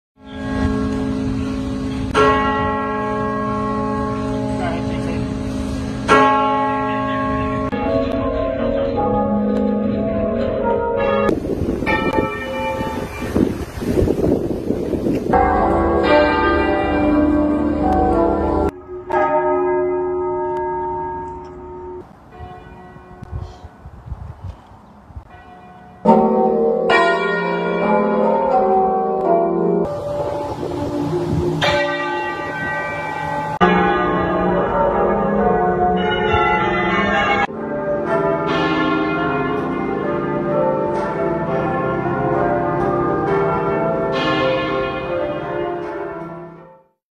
Regina Elisabetta, le campane di tutto il Regno Unito suonano per ricordarla
Dalle 12 chiese e parrocchie hanno dato vita a un "concerto a distanza" in omaggio alla sovrana